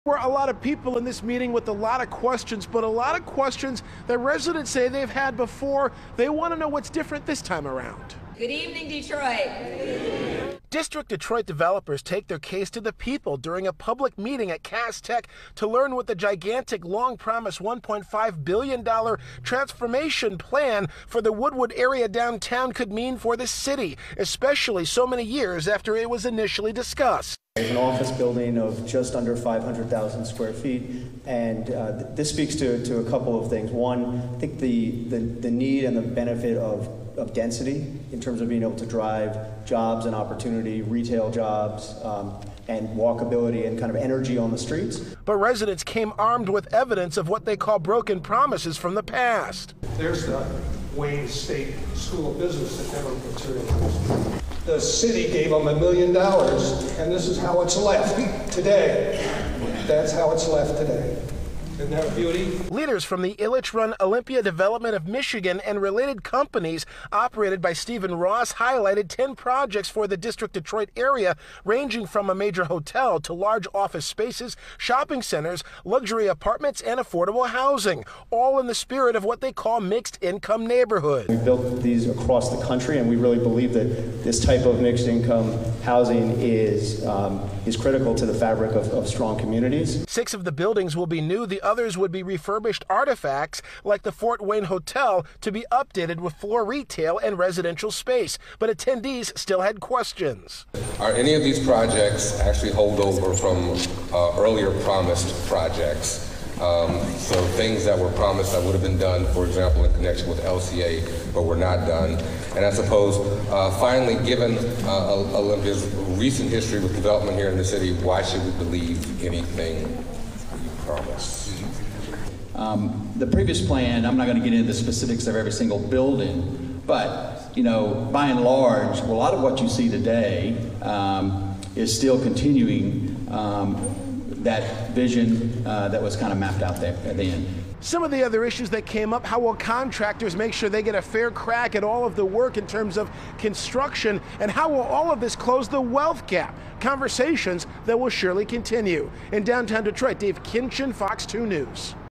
Detroiters not sold on latest pitch by Olympia Development for $1.5 billion downtown plan
District Detroit developers took their case to the people during a public meeting at Cass Tech High School to learn what the gigantic, long promised $1.5 billion plan for the Woodward-area could mean for the city, years after it was initially discussed.